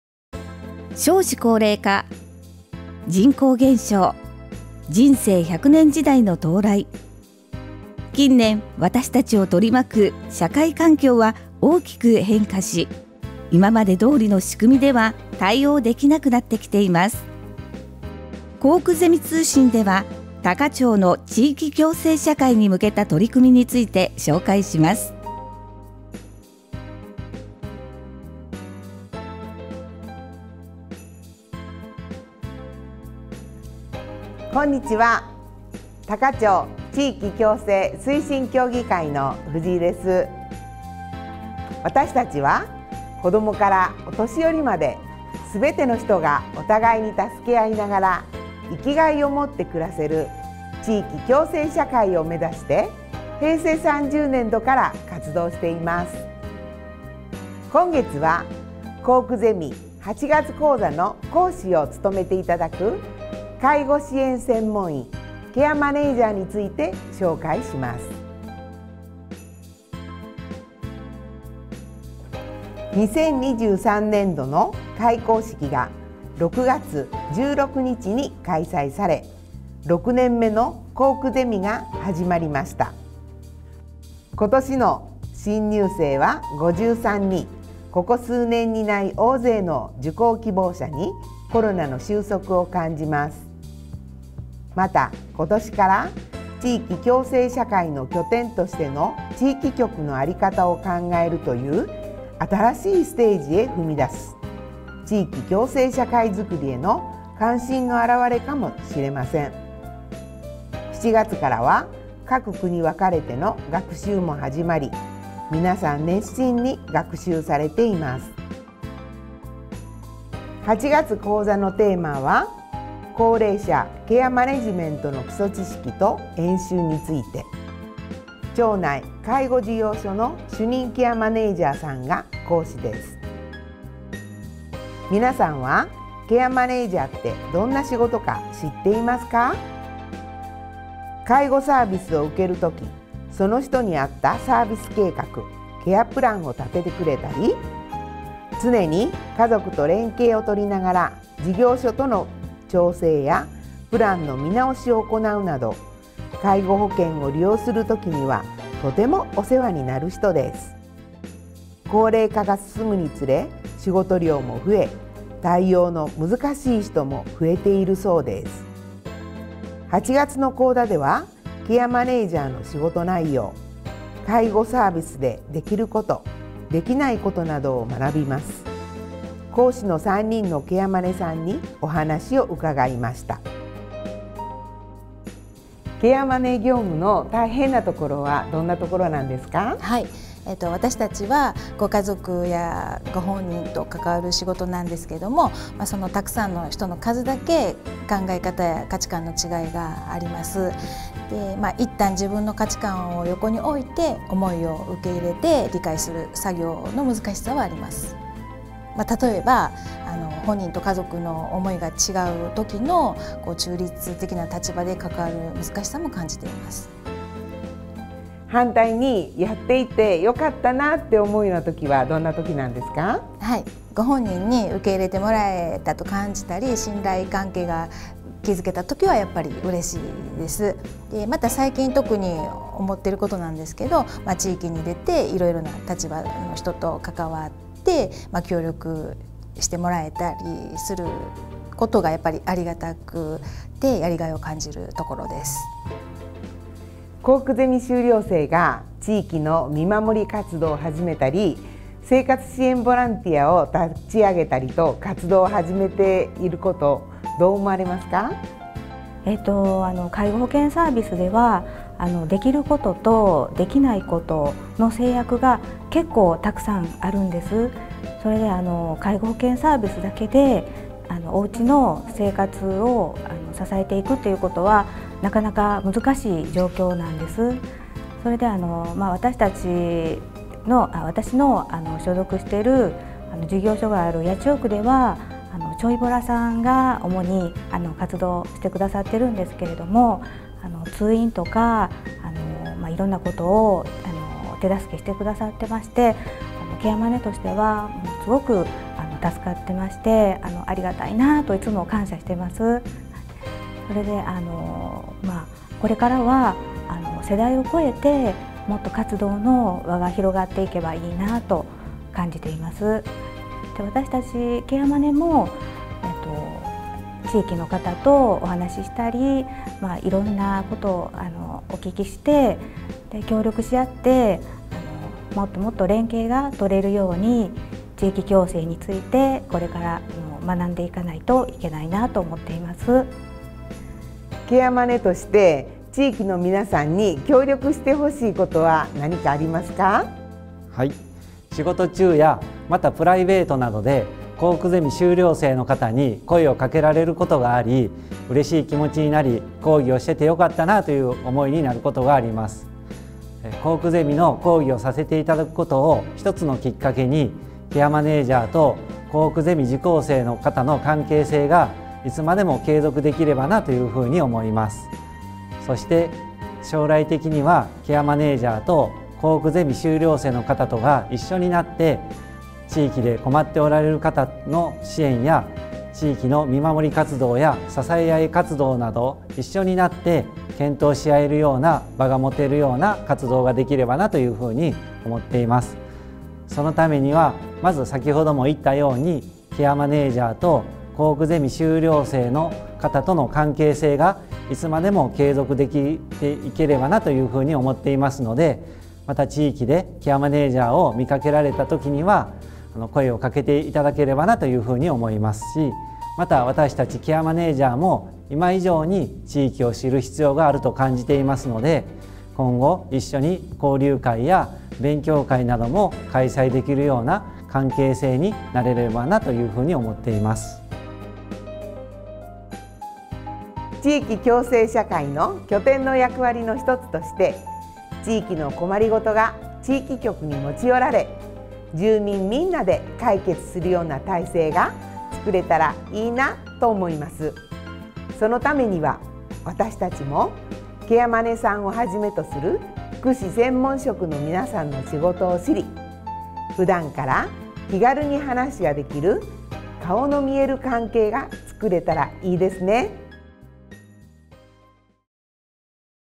コークゼミ通信（R5年7月号）
今回のコークゼミ通信は、ケアマネジャーのインタビューをお送りします。